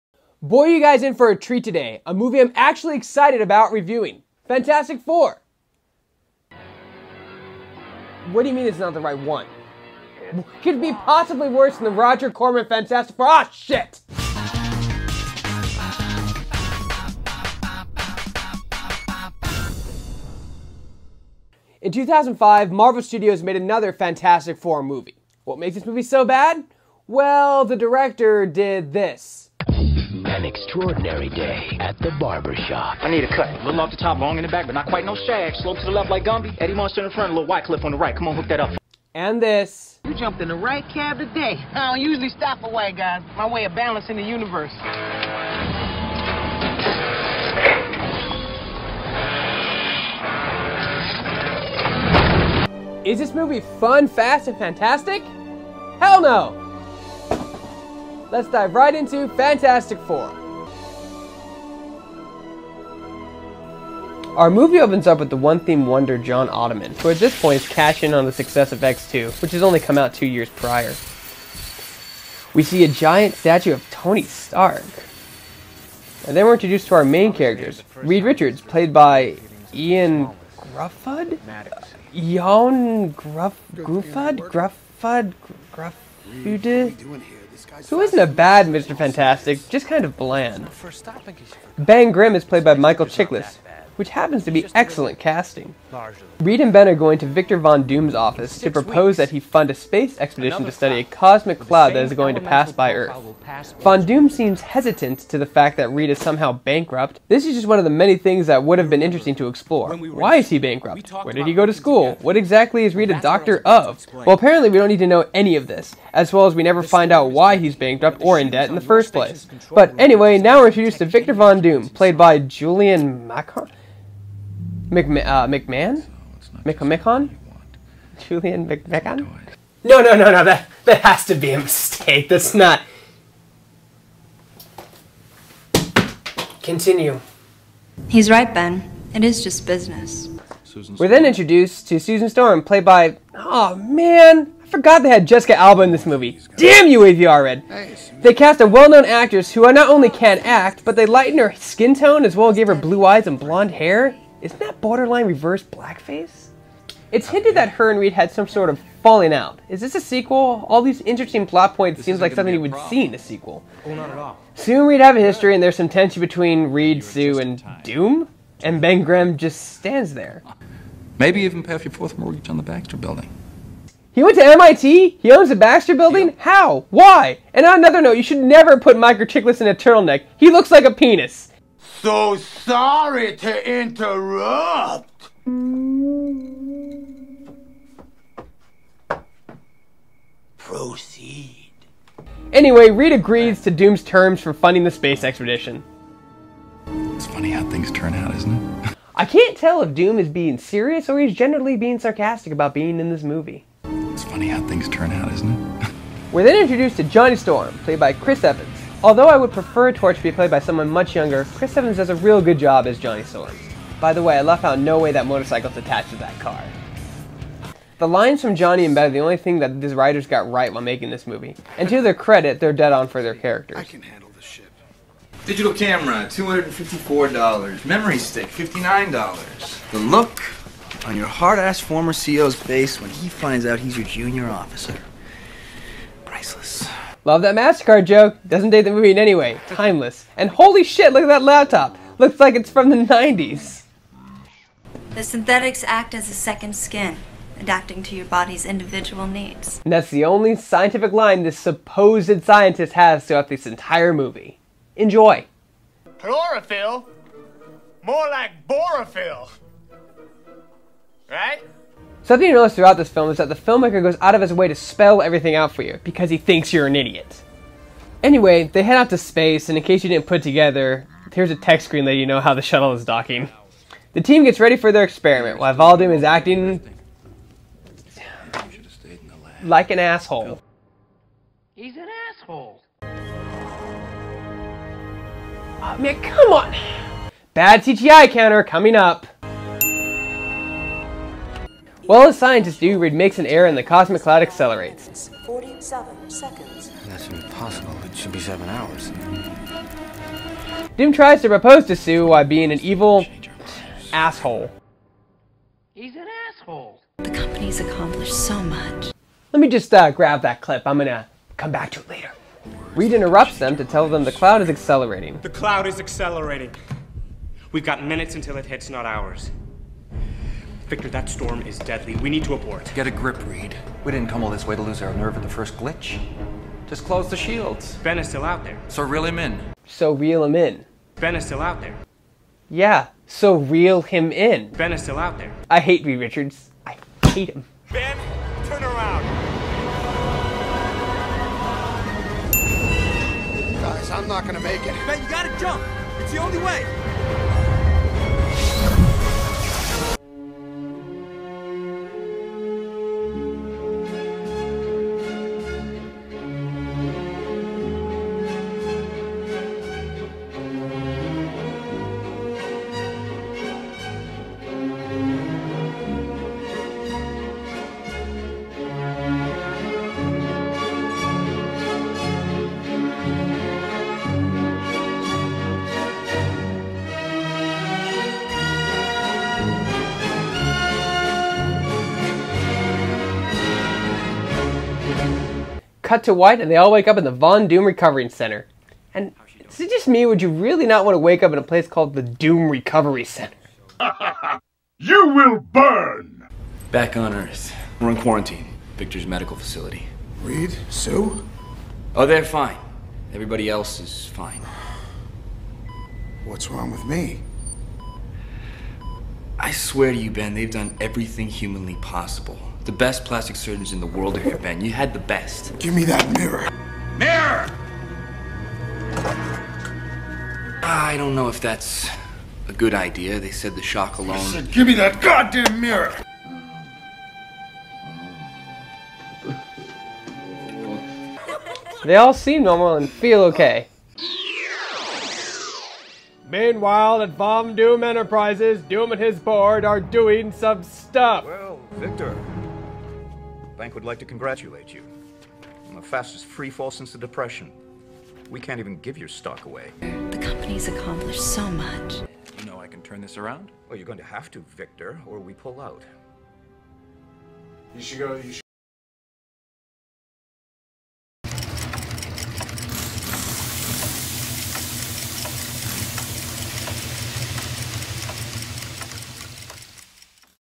The Fan Review: Fantastic Four
Its not the full review, I had a system crash and lost everything, Im giving you guys what I saved. Hope you enjoy it! Sorry for the multiple uploads, youtube kept flagging it.